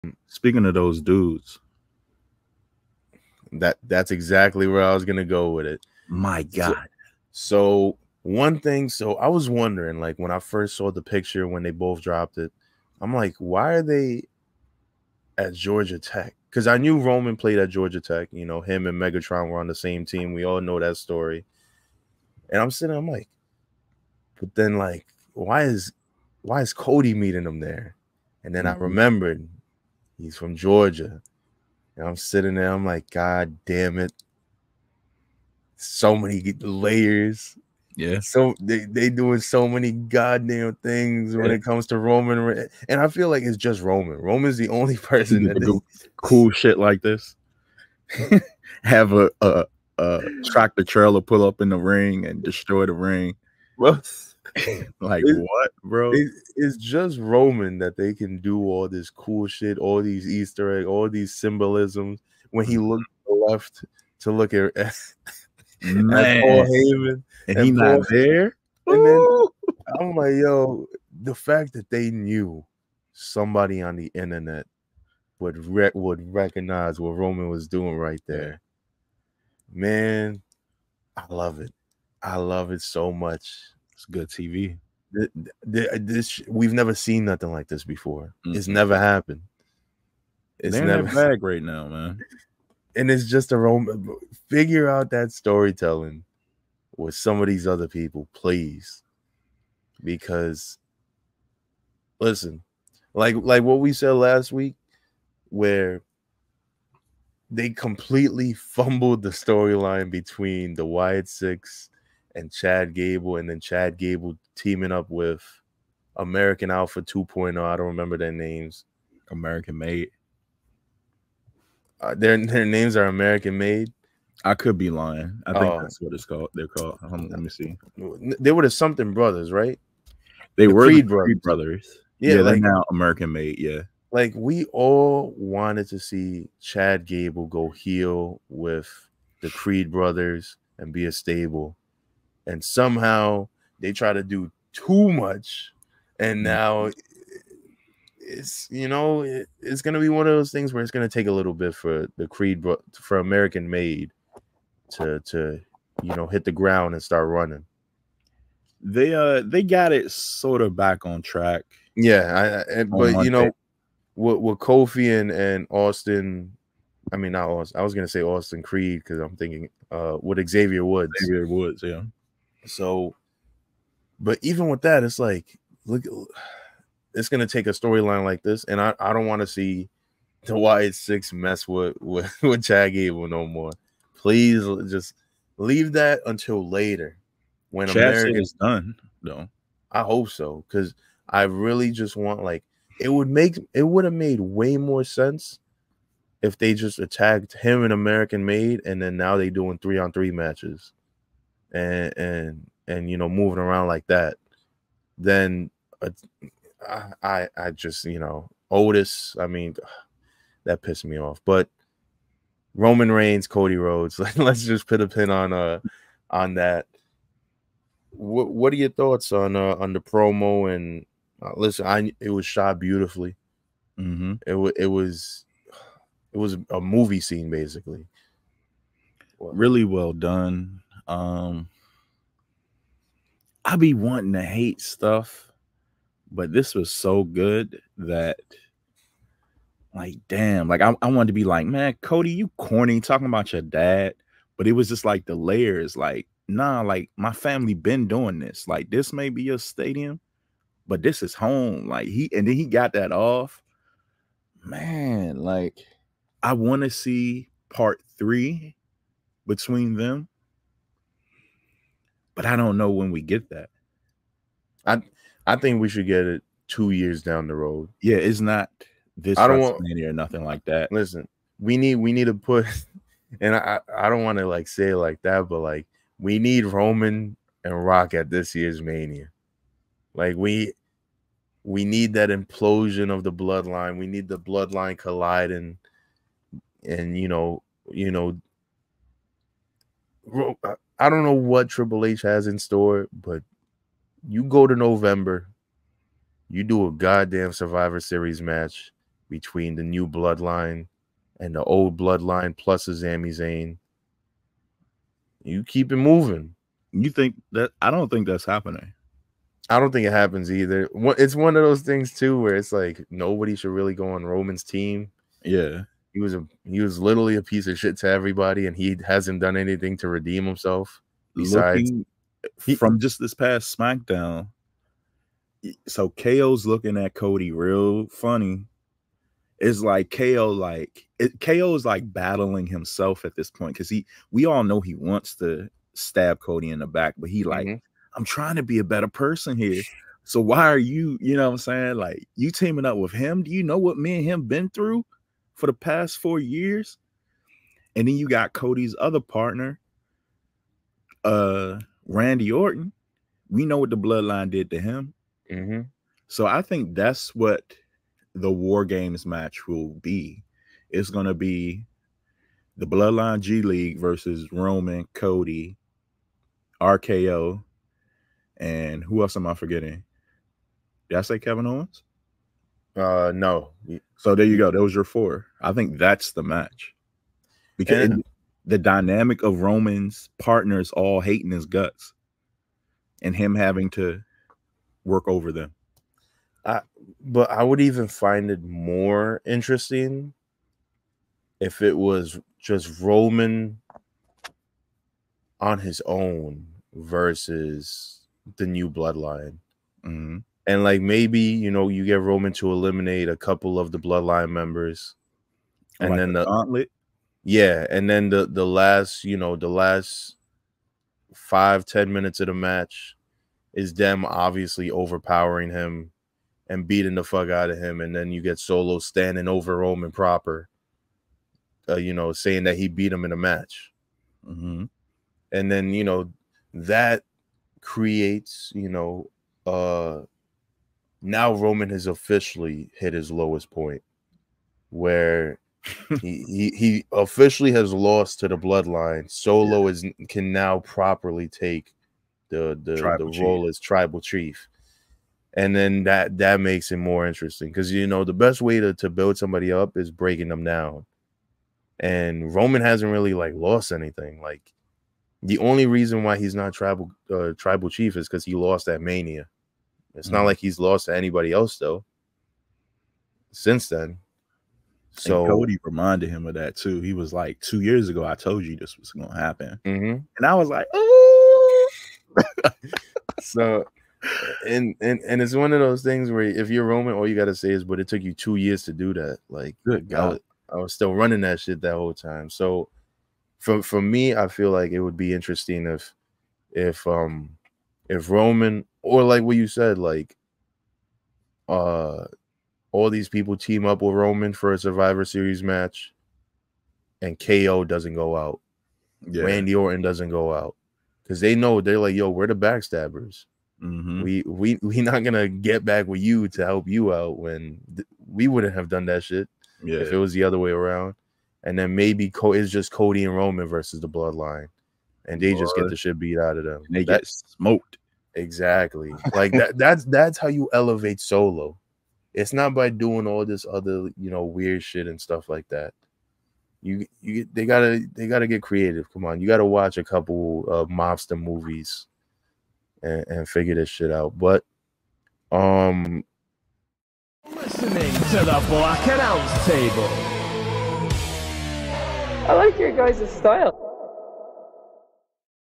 Dave Meltzer says this Cody Rhodes Roman Reigns Promo was produced by Paul Heyman
Listen to our podcast "The Black Announce Table" on all streaming platforms including Apple, Spotify, SoundCloud, etc.